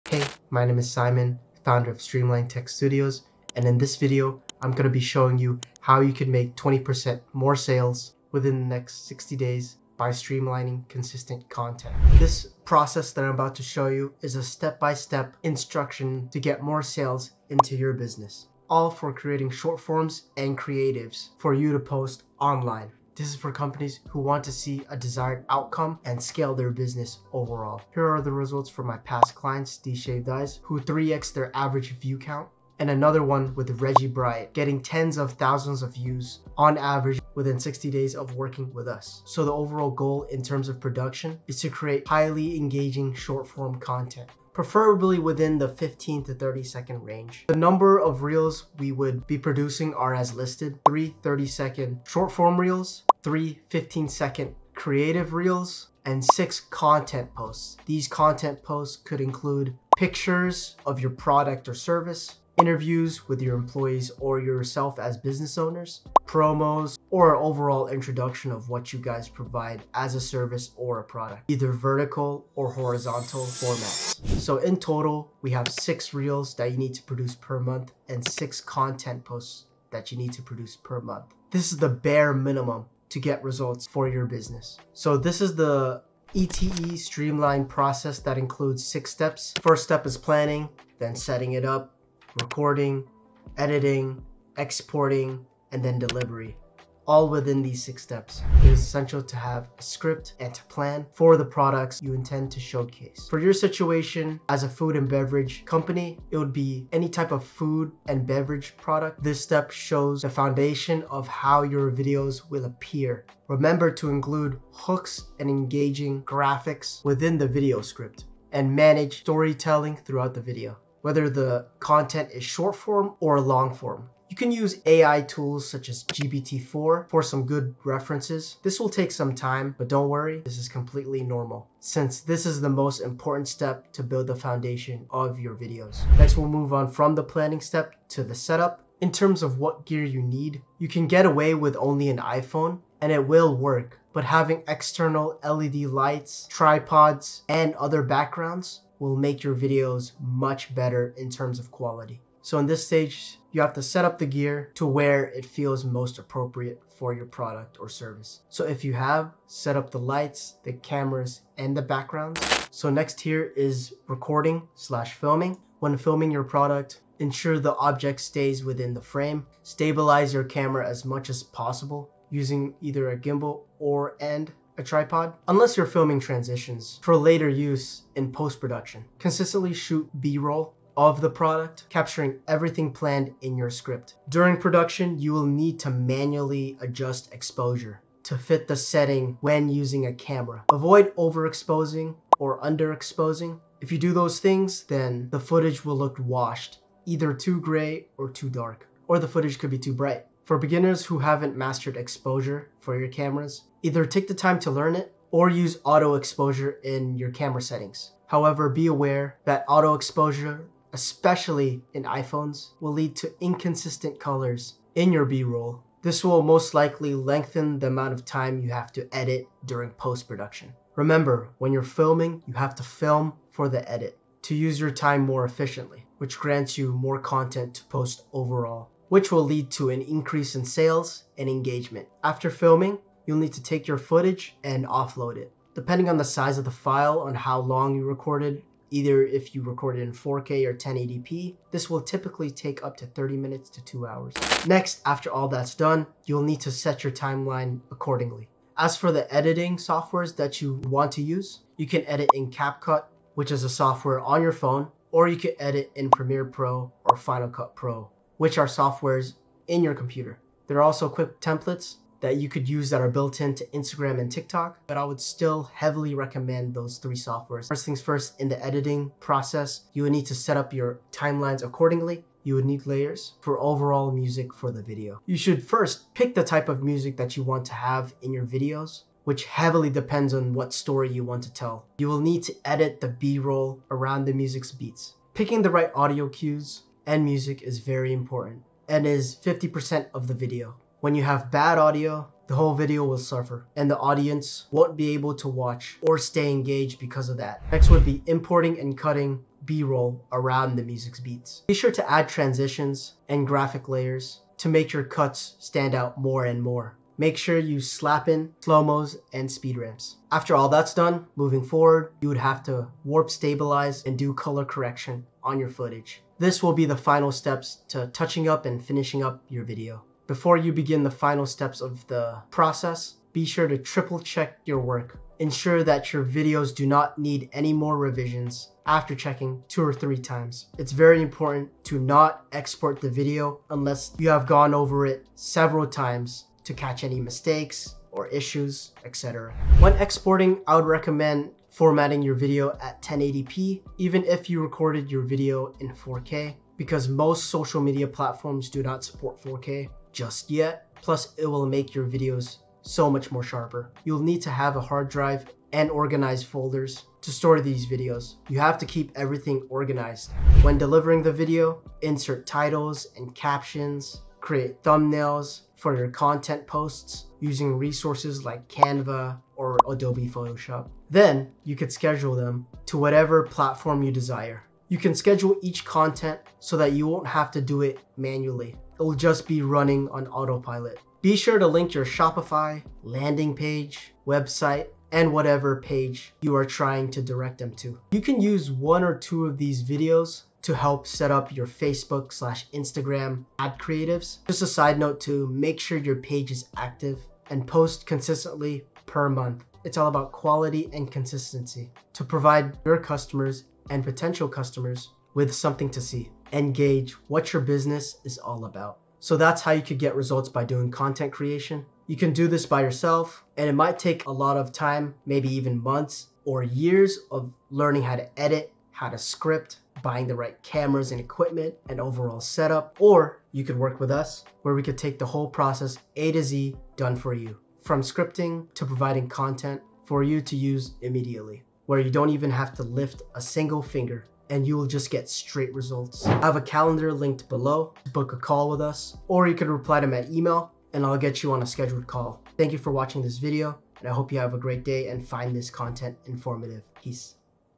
How Brands Can Achieve 20% Increase in Revenue in Under 60 Days
Here in this video, I explain the ETE process to get you more sales for your business.

If your business is interested in getting more revenue through video